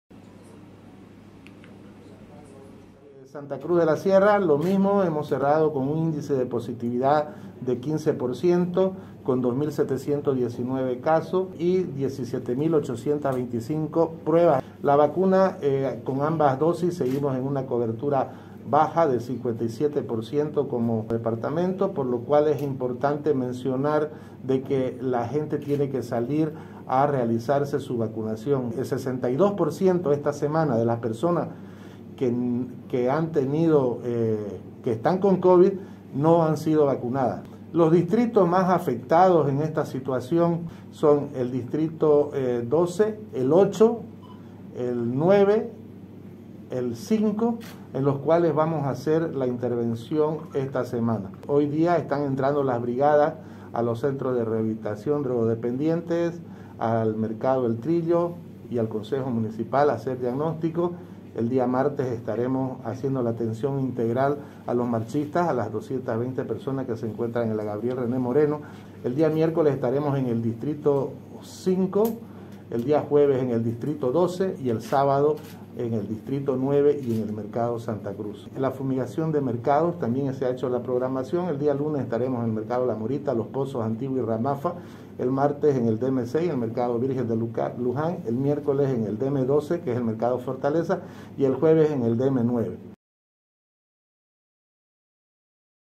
Nuevo informe sobre el Covid-19 en Santa Cruz de la Sierra
El Gobierno Autónomo Municipal de Santa Cruz de la Sierra brindó el informe epidemiológico en relación al Covid – 19, el cual muestra un ascenso en los casos. Se cerró la semana 47 con un índice de positividad de 15%, 2.719 positivos y 17.825 pruebas realizadas. Los distritos con más contagios son 12, 8, 9 y 5. Roberto Vargas, secretario de Salud indicó que se reforzarán todas las acciones por la salud, con el despliegue de brigadas médicas a los barrios y fumigación y desinfección en los mercados.